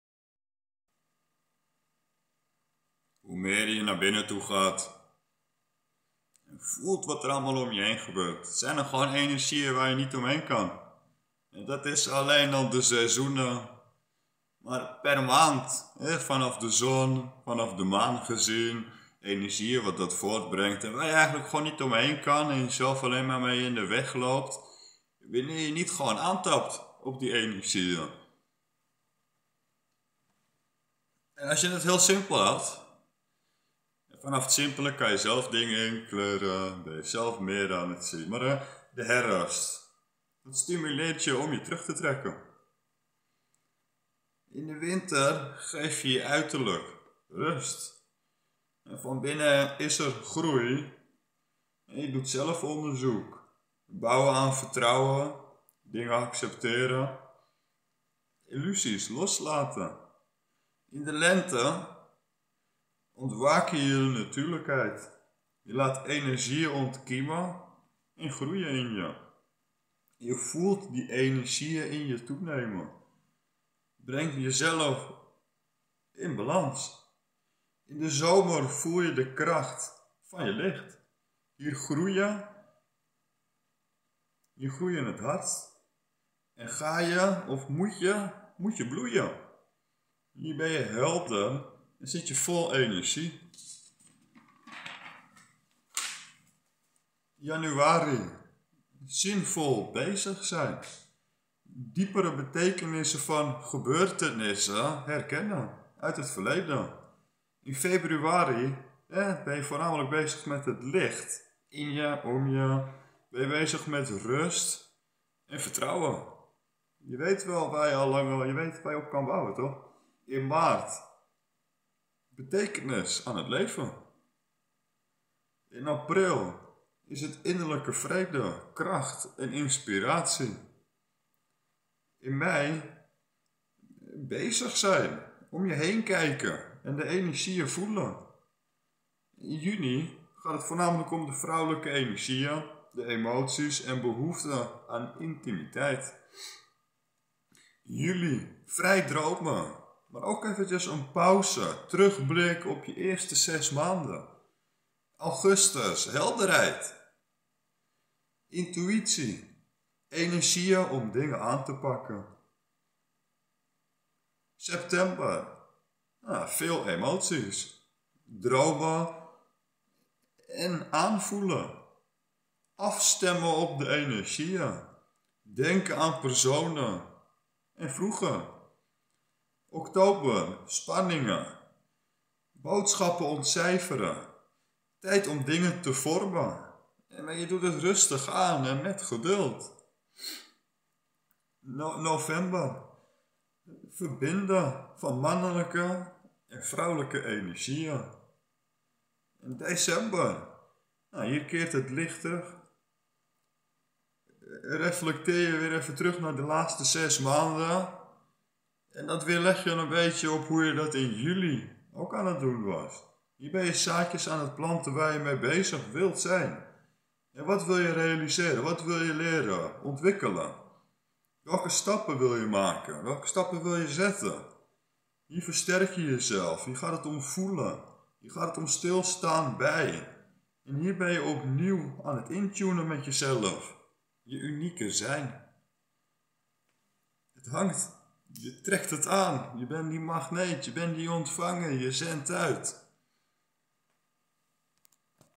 Hoe dieper je in jezelf gaat, hoe meer je de energie om je heen waarneemt | Mee met de flow..
DECEMBER OPENBAAR

Tegenstribbelen heb je niks aan, je moet met de energie mee... Rond rennen als een kip zonder kop, onzin! Andere volgen en je laten afleiden, brengt je nergens.

En tussen jouw en de energieën van de zon en maan (astrologie) ligt nog veel meer, neurologische gezien ;) Je zintuigen waarnemen, zijn niet verbonden met het alles, zoals je zenuwen ect dat zijn ;) Mond, body, soul dingen. wakker in de matrix is alles verbonden. xx